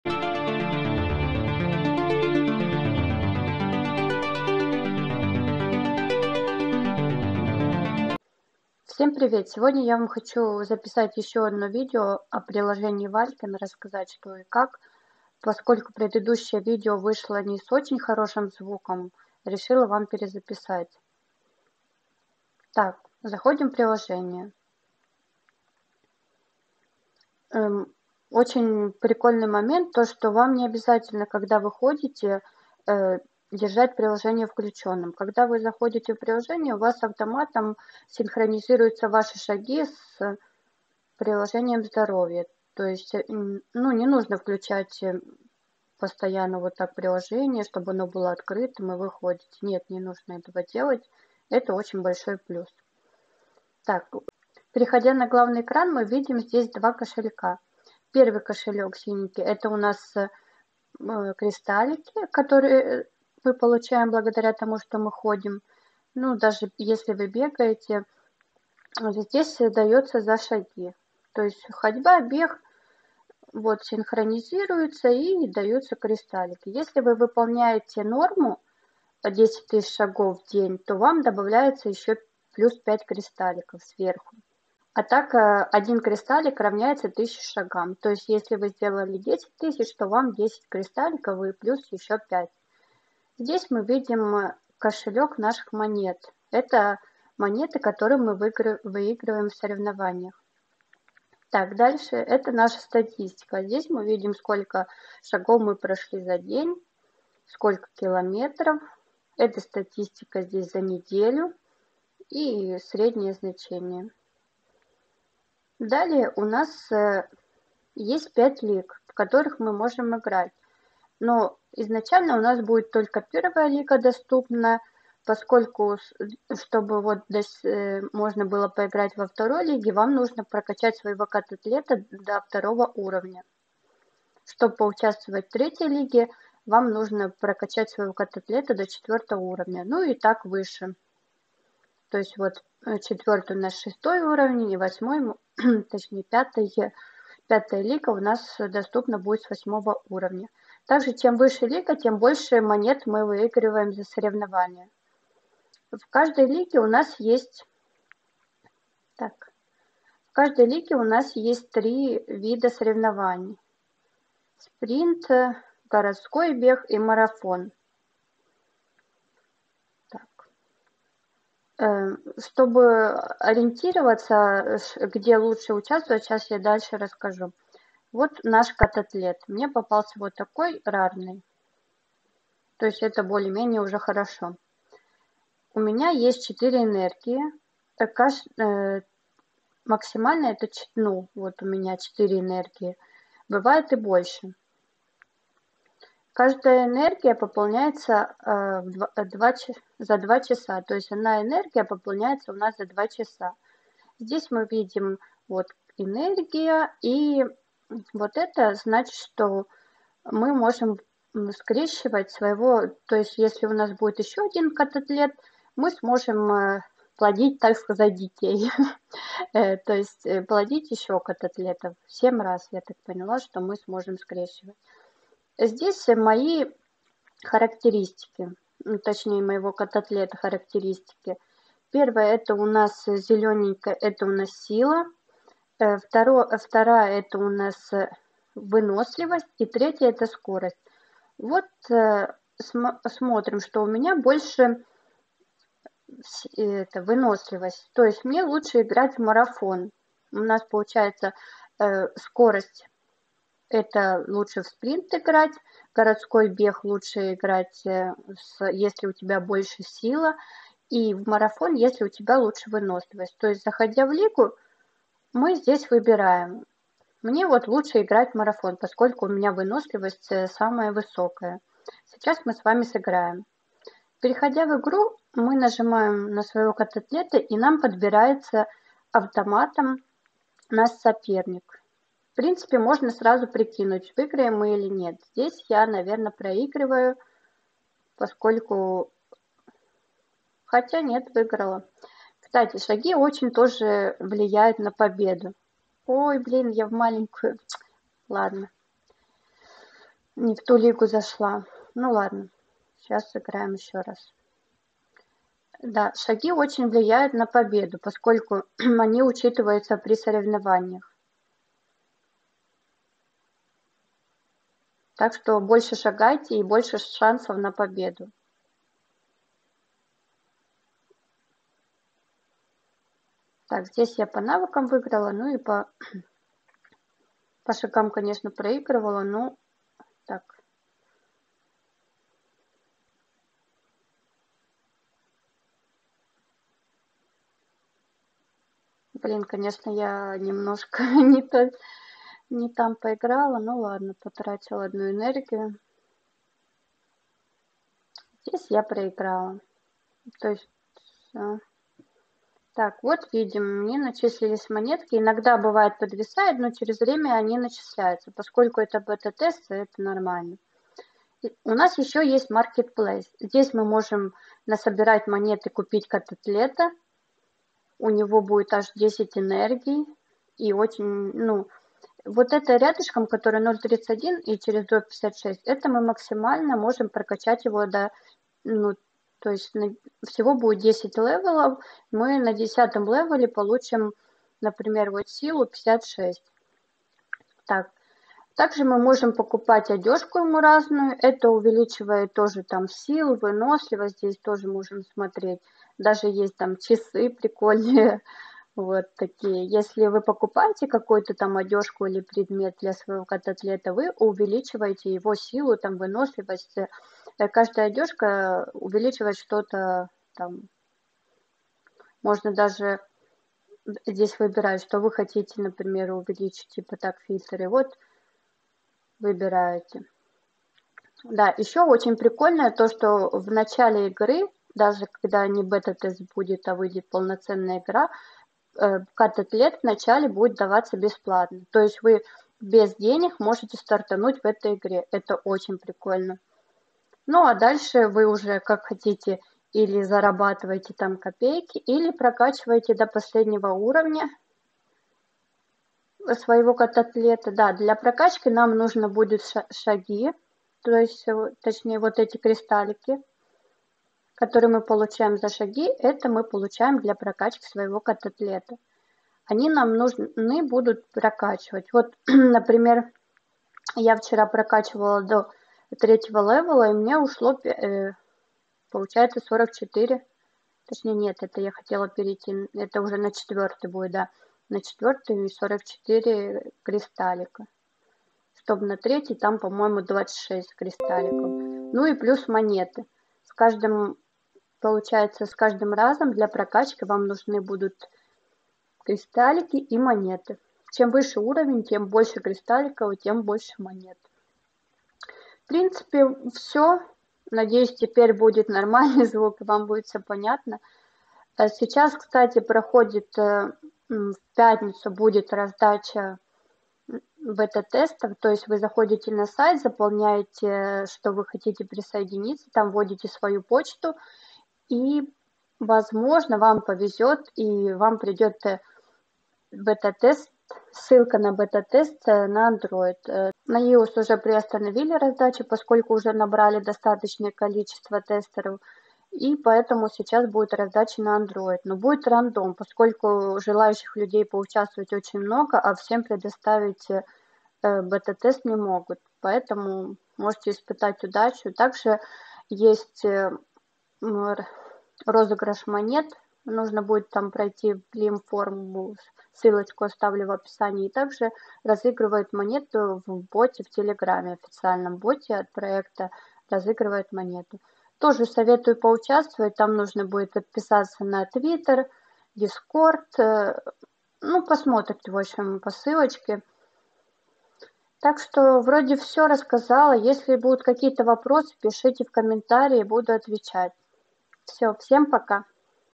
ОБЗОР ПРИЛОЖЕНИЯ WALKEN | ГУЛЯЙ И ЗАРАБАТЫВАЙ
Для тестирования нужно подать заявку на официальном сайте указав устройство, с которого планируешь тестировать. На iOS пока что набор приостановлен, но в дальнейшем должны еще запустить рассылку. На Android будет рандомная рассылка в эту пятницу. Следи за новостями 🙂

ответы на вопросы:
1 -1000
2 - Cat + Athlete
3 - 5
4 - Solana
5 - 5